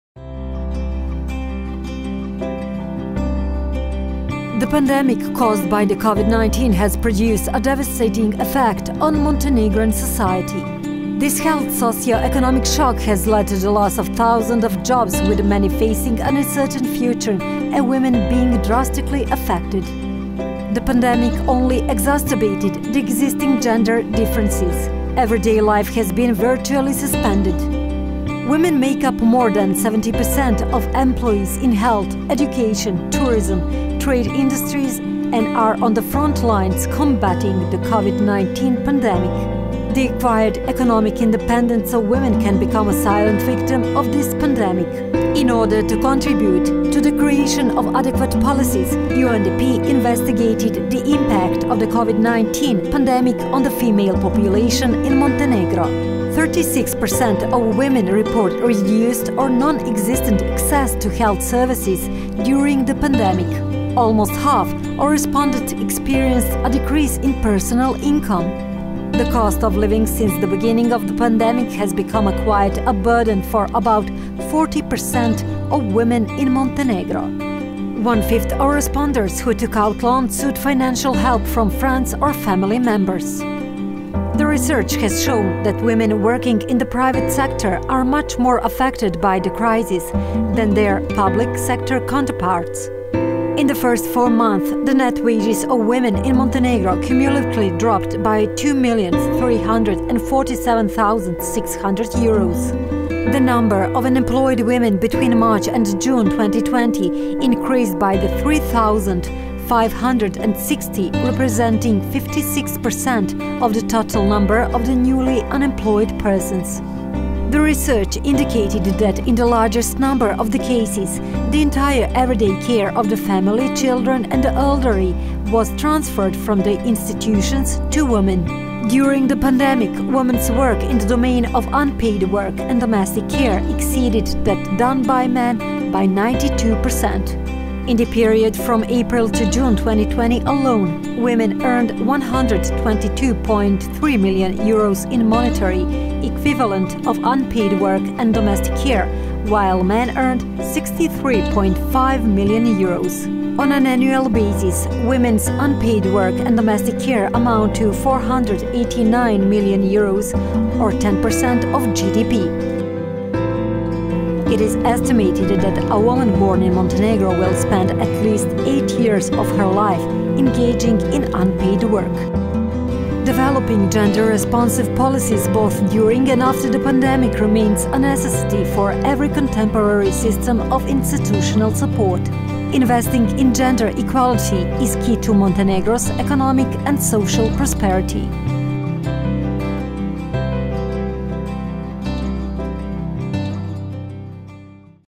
WOMEN’S CONTRIBUTION TO THE ECONOMY OF MONTENEGRO AND UTILISATION OF CARE ECONOMY
The pandemic caused by the COVID-19 virus had a devastating effect on Montenegrin society.
This health-socio-economic earthquake has led to the loss of thousands of jobs, and many are facing an uncertain future, among which women have been drastically affected.
The pandemic only increased the existing gender differences.